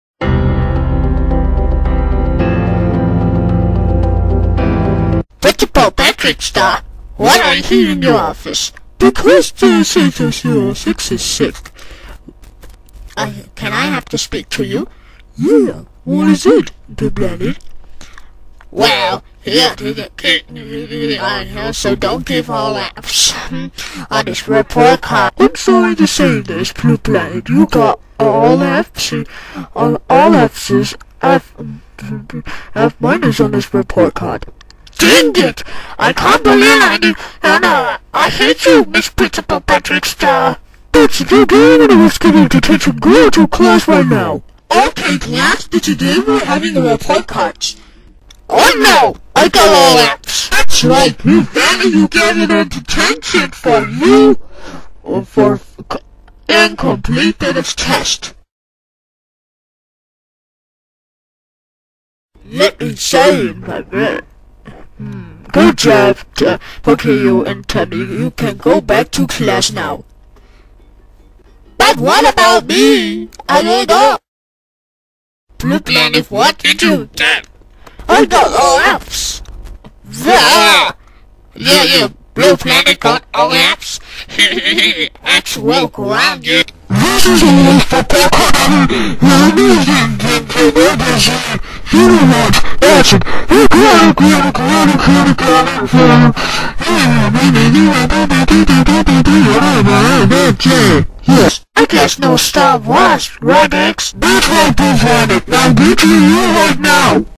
Blue Planet Fails A Test And Gets Detention And Grounded In G Major 18
Original Uploaded Date: September 18th, 2016